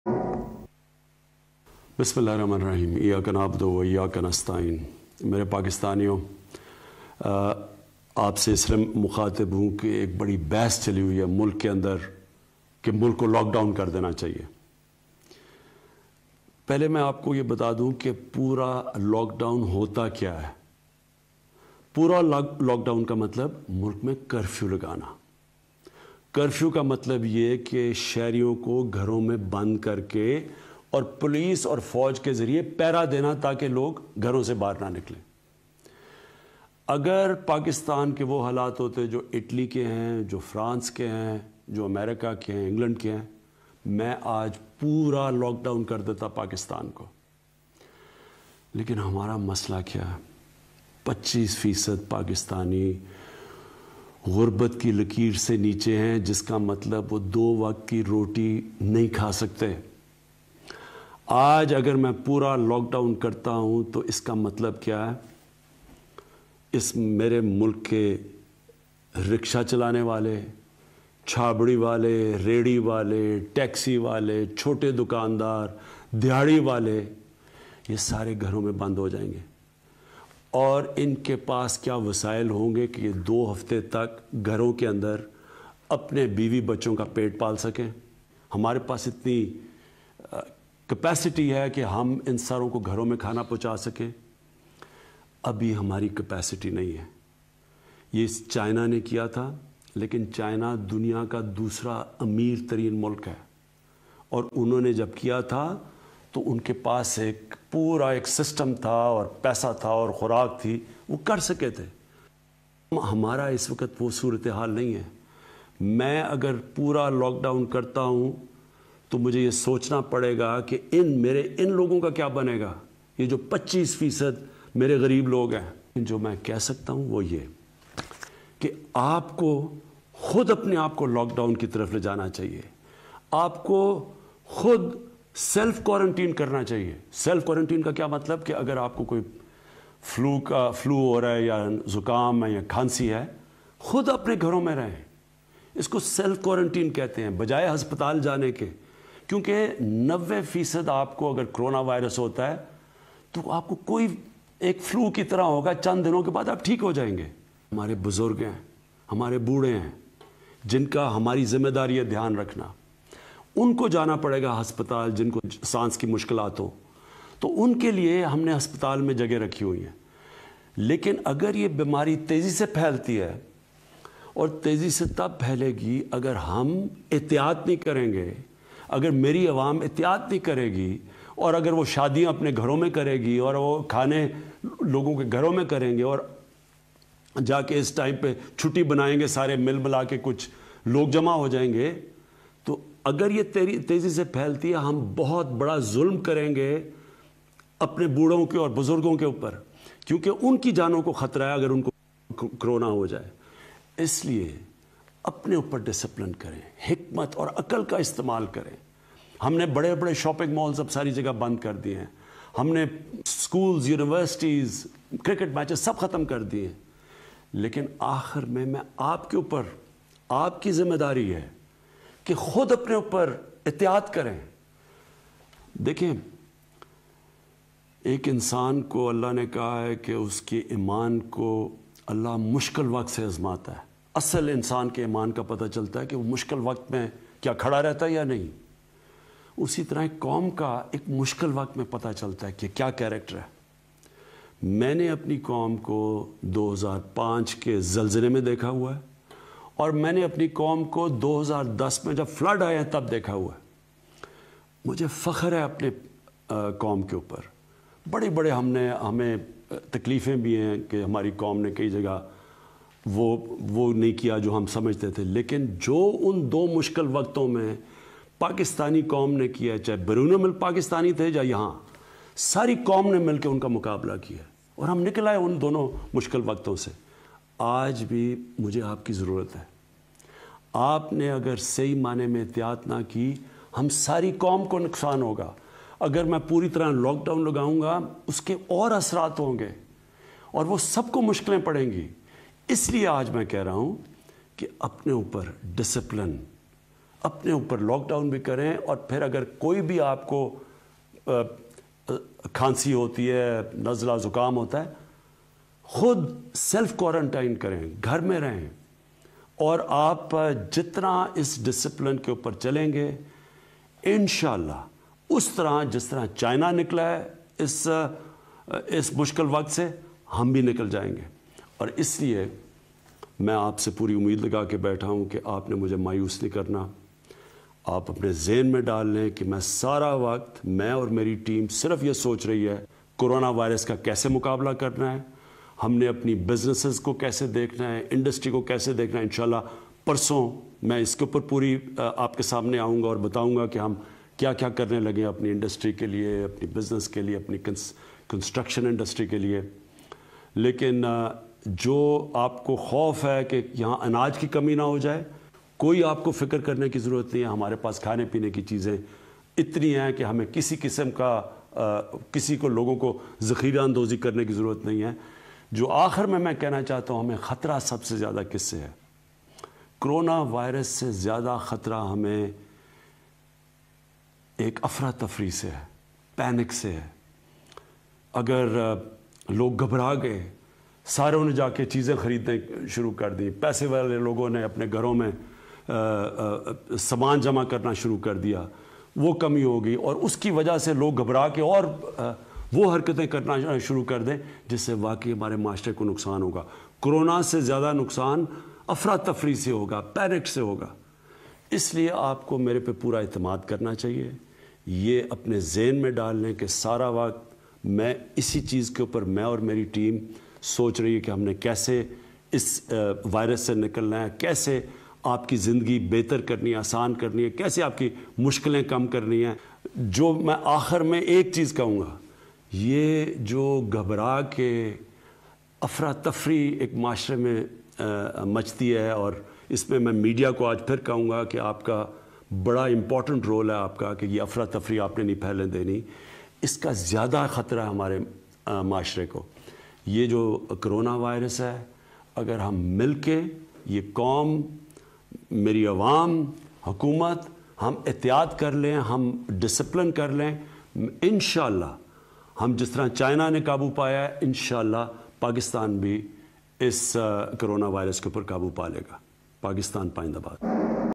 Prime Minister Of Pakistan Imran khan Today 22 March 2020 live Full Speech due to Coronavirus.
اگر ہمارے حالات اٹلی یا فرانس یا امریکہ جیسے ہوتے تو اس وقت میں پورا پاکستان لاک ڈاؤن کر دیتا لیکن ہمارا مثلہ یہ ہے کہ پچیس فیصد پاکستانی غربت کی لکیر سے نیچے ہیں جس کا مطلب ہے وہ دو وقت کی روٹی بھی نہیں کھا سکتے۔ آج اگر میں پورا لاک ڈاؤن کرتا ہوں تو اس کا مطلب ہے غریب لوگ گھروں میں بند ہو جایٔں گے اور کیا ان کے پاس وسایٔل ہونگے کے دو ہفتے کے لیۓ گھر بیٹھ کر اپنے بیوی بچوں کو دو وقت کی روٹی دے سکیں ۔
ابھی ہماری کپیسٹی نہیں ہے کہ ہم ان سب کو گھروں میں کھانا پہنچا سکیں
پاکستان کی اعوام مکمل لاک ڈاؤن نہیں برداشت کرسکتی
ہمیں خود اپنے آپ کو لاک ڈاؤن کرنا ہوگا
اگرلوگوں کو گھر میں بند کردیا تو مزدور کیا کرے گا